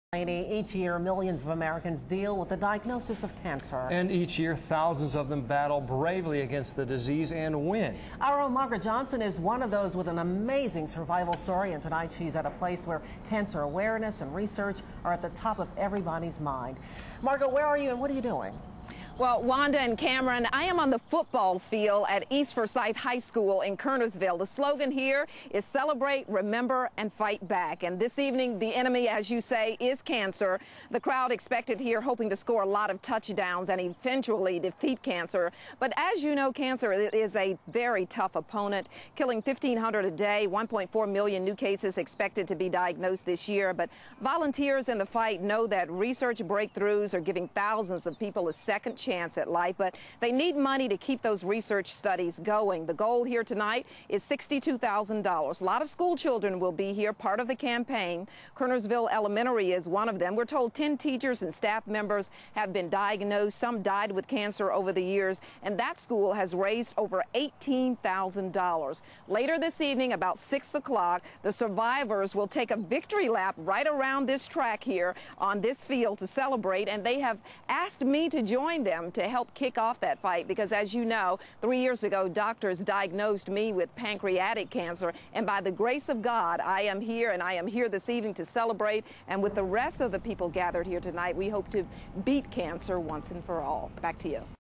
Relay For Life Kicks Off In Kernersville
WXII's Margaret Johnson is taking part in this year's event.